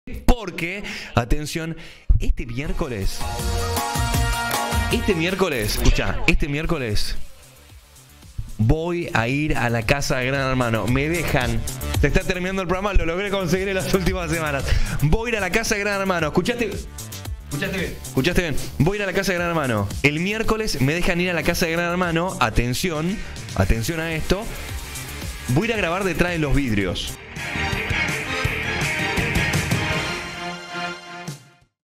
Poggi En La Casa
¡Se le dio! 🤩 @poggi está emocionado por la llegada de este miércoles especial que compartirá en el IRLTelefe